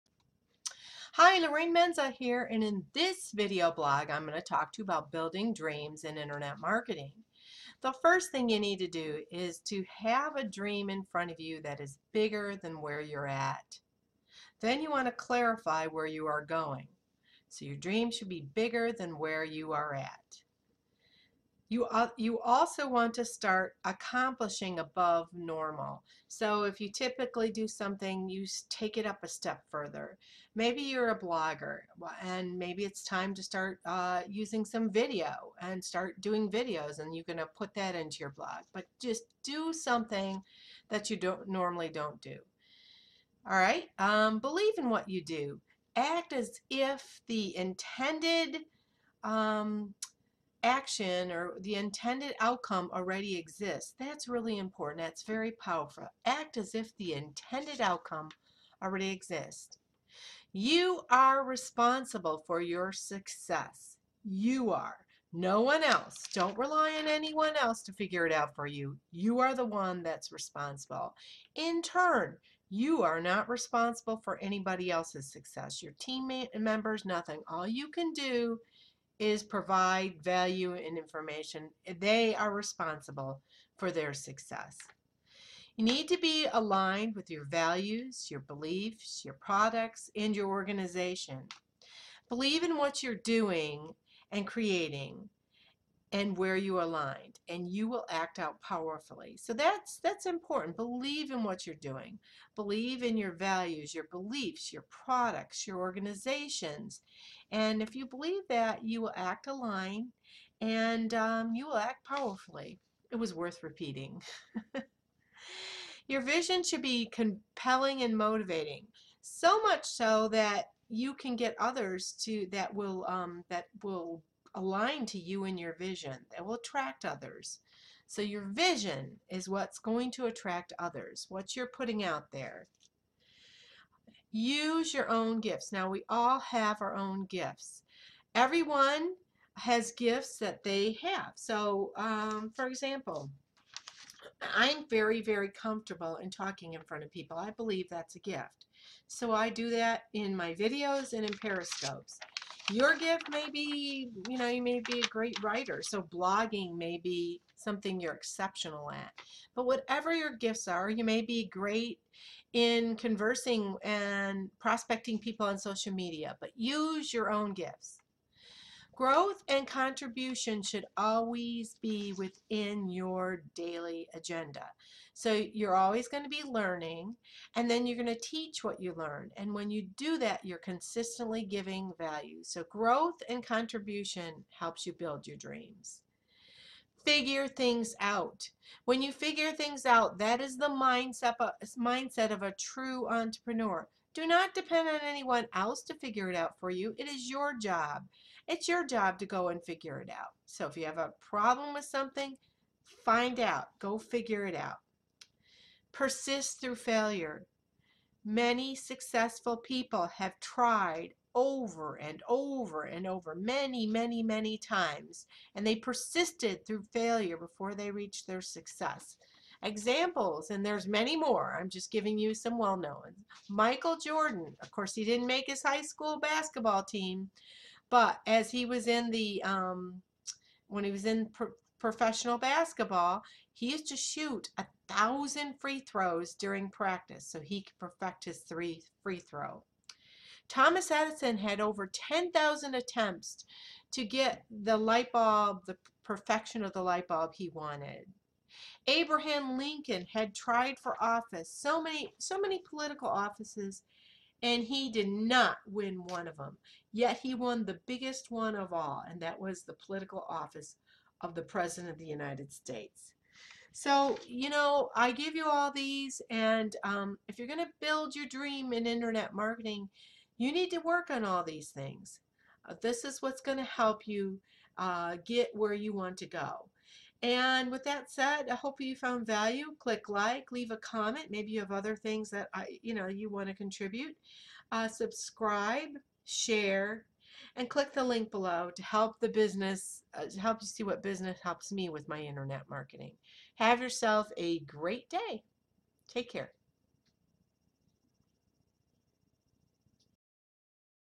Building Dreams in Internet Marketing
Click the link to the left

Hi, Lorraine Menza here and in this video/blog I am going to address building dreams in internet marketing.  We all have dreams; but how do we accomplish these dreams?

• Have a dream in front of you daily that is bigger than where you are at.  Clarify where you are going!

• Start accomplishing above normal.

• Believe in what you do.  Act as if the intended outcome already exists.

• You are only responsible for you success.  No one else is responsible.  You are taking the correct actions.  You are not responsible for other people’s successes.  They are responsible for their success.

• Be aligned with your values, beliefs, your products, and your organization.  Believe in what you are doing and creating.  When you are aligned, you will act powerfully.

• Your vision should be compelling and motivating that will attract others to you and your vision.

• Use your own gifts.  Everyone has special gifts that they can use in their internet marketing.

• Growth and contribution are two things that should always be happening in your marketing.  Always be learning and then teach what you learn to others.  You will be consistently be giving value!

• Figure things out!  That is the true mindset of an entrepreneur!  Be willing to learn and figure things out.  Don’t rely on others to figure it out for you!

• Persist through failure.  Many successful people had to try many, many times to produce success.  Be one of those people.  Michael Jordan used to practice 1000 free throws in practice.  Thomas Edison has 10,000 attempts before he got the light bulb perfected.  Abraham Lincoln ran for many different political offices and never won; all except the biggest office ever, the presidency of the United States!